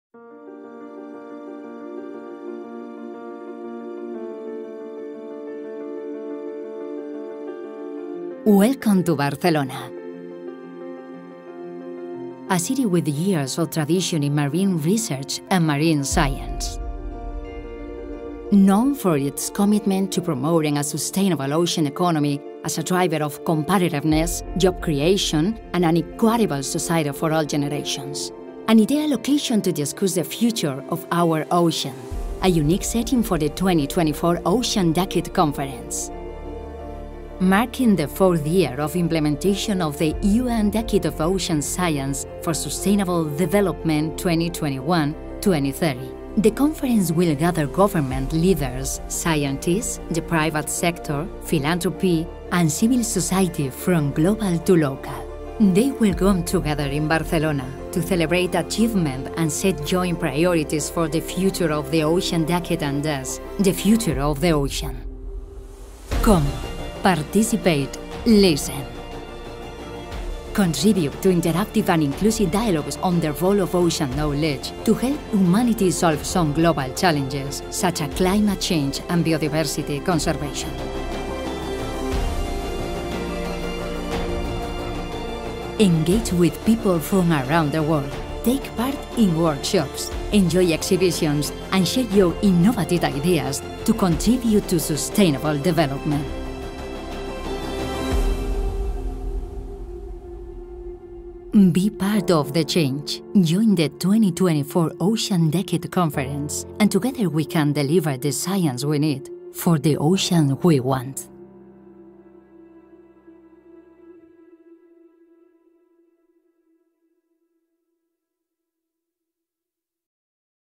2024 Ocean Decade Conference - Presentation (English)
2024 will mark the fourth year of implementation of the UN Decade of Ocean Science for Sustainable Development (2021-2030). Under the leadership of UNESCO’s Intergovernmental Oceanographic Commission, the Ocean Decade has already galvanized thousands of partners around the world to start delivering the science we need for the ocean we want.

The time is now ripe to address key questions related to the Ocean Decade: What have been the main achievements since the launch of the Ocean Decade? What should be the key areas for action over the next three to five years?

Focused on ‘delivering the science we need for the ocean we want’, the 2024 Ocean Decade Conference, which will be hosted by the Government of Spain in Barcelona from 10 - 12 April 2024, will bring together the global Ocean Decade community and partners to celebrate and take stock of progress, and set joint priorities for the future.